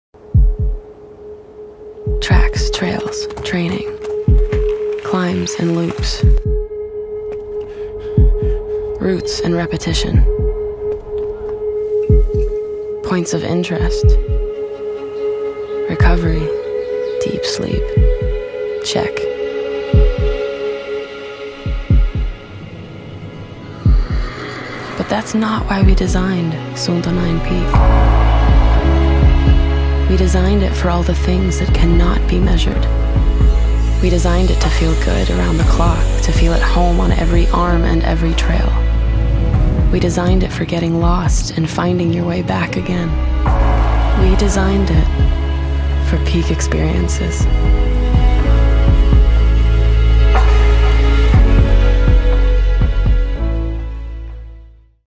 BOMAR SUUNTO 9 Peak
Dizajniran za vrhunska iskustva koje vam sport može pružiti, i sve avanture koje će te zauvek pamtiti. SUUNTO 9 PEAK je najtanji, najmanji i najotporniji sat koji je Suunto ikada napravio.

Suunto 9 Peak koristi grade 5 titanijum koji ima manju težinu i pruža vrhunsku udobnost. Titanijum je tvrd, otporan na grebanje i hipoalergenski materijal, što je najbolji izbor za vašu kožu. Vrhunski izgled i osećaj koji se ne da izmeriti.

Najtanji, najmanji i najotporniji sat koji je Suunto ikada proizveo će vas dovesti do vrhunskog iskustva. Od 25 sati trajanja baterije sa najzahtevnijim GPS-om do 170 sati u Tour modu. Materijali vrhunskog kvaliteta, safirno staklo i grade 5 titanijum za malu težinu. Premium kavlitet napravljen u Finskoj i testiran u najtežim uslovima.

Za puteve na kojima hoćete da se izgubite i nađete svoj put nazad. Iskusite najzanimljivije lokacije sa Point of Interest (Poi) sa Suunto app. Planirajte vaše rute na Suunto app sa sport-specific heatmaps i popularnim startnim tačkama. Suunto 9 Peak je vodootporan do 100m, napravljen da izdrži sve avanture.

Povežite Suunto 9 sa Suunto app da pratite sve vaše avanture, kao i da pratite vaše dugoročne trendove, ukljucujuci dnevne aktivnosti i praćenje sna. Lako podelite vaše najveće dostignuće i povežite se sa ostalima iz Suunto zajednice. Proverite vaše dolazne pozive, poruke i notifikacije jednim pogledom na vaš sat. Suunto app je dostupan na App Store i Google Play.